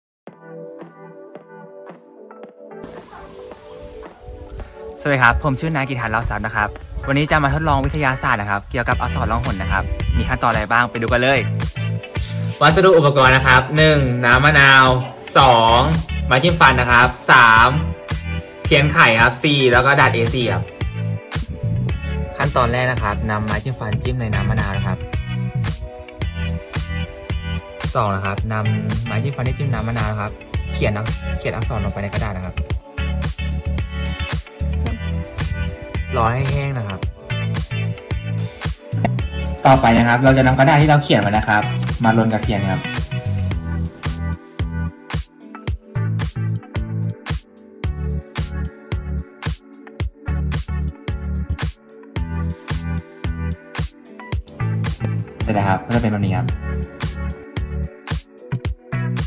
การทดลองตัวอักษรล่องหน
การทดลอง ผลงานนักเรียน โรงเรียนคุรุราษฎร์รังสฤษฏ์ สารคดีสั้น